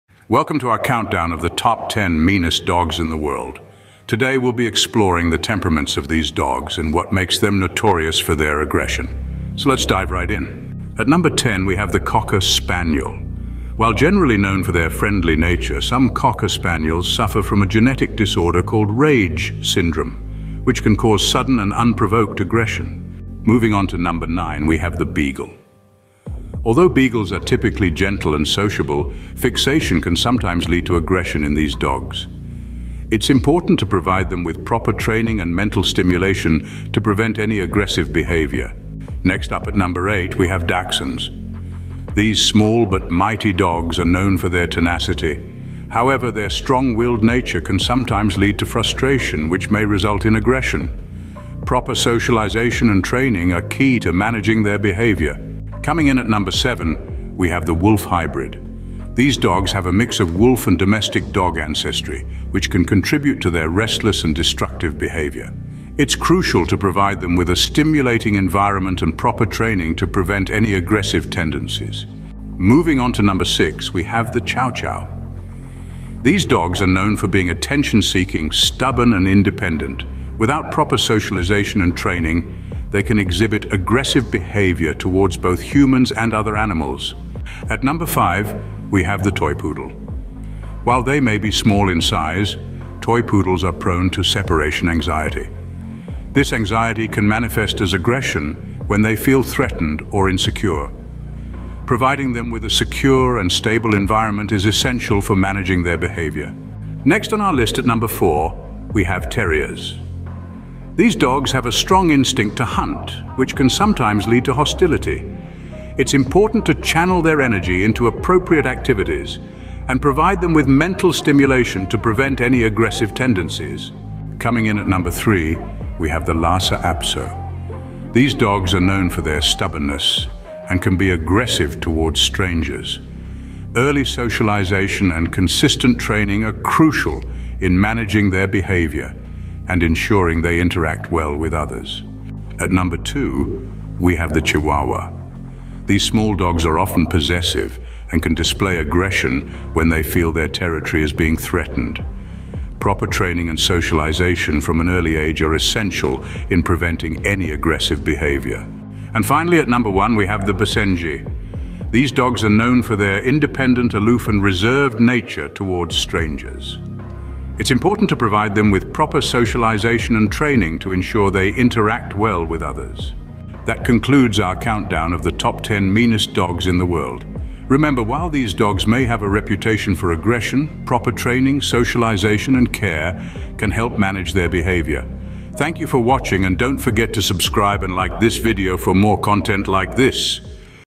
Top 10 Meanest Dogs In The World | Animals Majesty
🐾 *Discover the Surprising Side of Man’s Best Friend! | Top 10 Meanest Dogs in the World* 🐾

Welcome to our latest video where we dive deep into the world of the most intimidating canine companions! Whether you’re a dog lover, a curious viewer, or a pet parent looking to understand more about different dog temperaments, this video is a must-watch! We’ve gathered insights and surprising facts about some of the dog breeds that are often labeled as "mean."

🔥 *Featured Breeds:*
Cocker Spaniel - Not just a pretty face!
Beagle - Small but mighty.
Dachshund - Courageous little warriors.
Wolf Hybrid - Wild at heart.
Chow Chow - Aloof and majestic.
Toy Poodle - Small dog, big attitude.
Terriers - Feisty and energetic.
Lhasa Apso - Small, with a lion's heart.
Chihuahua - Tiny but fierce.
Basenji - The barkless beauty with a bold temper.

🎥 *Why Watch?*

Surprising Behaviors: Discover the unexpected traits that make these breeds stand out.
Training Tips: Learn how to handle and train more challenging dog breeds.
Real-life Stories: Hear anecdotes from dog owners and experts.
This video isn’t just about showing which dogs have a mean streak; it's about understanding the unique traits and environments that influence their behaviors. So, whether you're considering adopting one of these breeds or just want to learn more about them, tune in!

*OUTLINE:*
00:00:00 Introduction
00:00:10 Number 10 - Cocker Spaniel
00:00:23 Number 9 - Beagle
00:00:38 Number 8 - Dachshunds
00:00:55 Number 7 - Wolf Hybrid
00:01:12 Number 6 - Chow Chow
00:01:27 Number 5 - Toy Poodle
00:01:46 Number 4 - Terriers
00:02:04 Number 3 - Lhasa Apso
00:02:22 Number 2 - Chihuahua
00:02:38 Number 1 - Basenji
00:02:53 Conclusion

👍 Like if you find our furry friends fascinating!